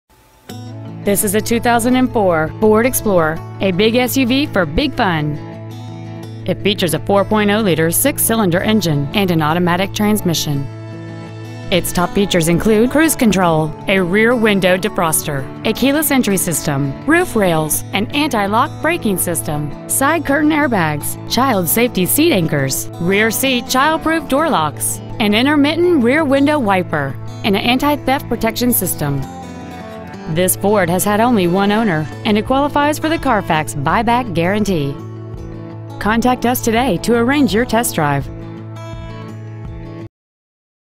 2004 Ford Explorer Phoenix AZ
We've been honored to serve the Phoenix AZ area , we promise that your experience at our dealership will exceed your expectations !

 Year : 2004
 Make : Ford
 Model : Explorer
 Engine : 4.0L (245) SOHC SEFI V6 FFV engine
 Trans . : Automatic
 Exterior : Other
 Miles : 110,176
 Interior : Other

 Preowned 2004 Ford Explorer Phoenix AZ